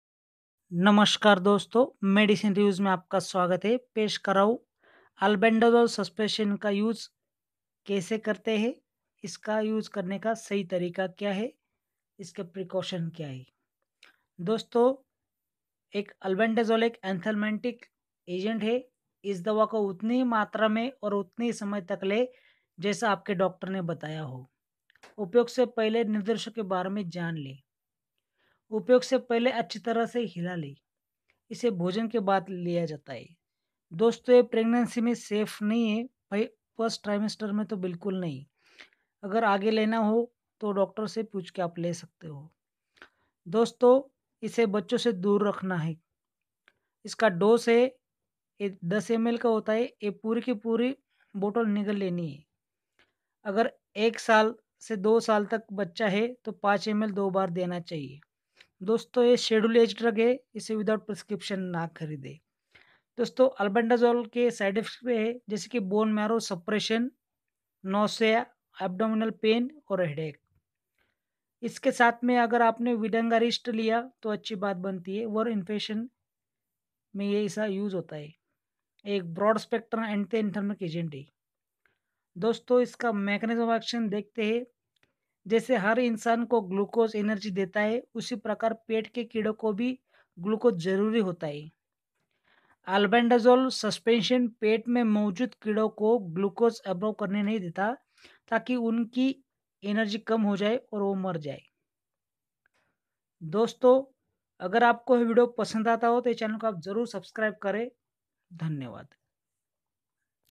Albendazole suspension review precaution, dosage & how to use
Albendazole suspension कैसे काम करता है
एल्बेन्डाजोल, एंटी-हेलमिन्थिक नामक दवाओं की श्रेणी से सम्बन्ध रखता है। एल्बेन्डाजोल, पेट में मौजूद कीड़ों को शुगर (ग्लूकोज) अवशोषित करने से रोकता है ताकि उनकी ऊर्जा कम हो जाय और वे मर जाएँ।
Albendazole suspension का इस्तेमाल कैसे करें
इस दवा को उतनी ही मात्रा में और उतने ही समय तक लें जैसा आपके डॉक्टर ने बताया है। उपयोग से पहले निर्देशों के बारे में जानने के लिए लेबल देखें। इसे मेजरिंग कप से लें और पी जाएं। उपयोग से पहले अच्छी तरह हिला लें। इसे भोजन के साथ या उसके बिना लिया जा सकता है लेकिन Albendazole suspension को एक निश्चित समय पर लेना बेहतर होता है।
गर्भावस्था के दौरान Albendazole suspension का इस्तेमाल करना असुरक्षित हो सकता है।
जानवरों पर किए गए अध्ययनों से पता चला है कि गर्भस्थ शिशु पर प्रतिकूल प्रभाव पड़ता है, हालाँकि, सीमित मानव अध्ययन किए गए हैं। गर्भवती महिलाओं में इसके इस्तेमाल से होने वाले फायदे, इसके जोखिम के बावजूद स्वीकार किए जा सकते हैं। कृपया अपने डॉक्टर की सलाह लें।
Albendazole suspension 200mg स्तनपान के दौरान उपयोग करने के लिए सुरक्षित है। मानव अध्ययन से पता चला है कि या तो यह दवा महत्वपूर्ण मात्रा में स्तन के दूध में प्रवेश नहीं करता है या इससे बच्चे में विषाक्तता पैदा होने की उम्मीद नहीं है।
किडनी रोग के रोगियों में Bandy 200mg Suspension के उपयोग पर सीमित जानकारी उपलब्ध है। कृपया अपने डॉक्टर से सलाह लें। onlinemedicine,medicine,ayurveda,ayurved ,Ayurvedic Instagram id vijaylahore4262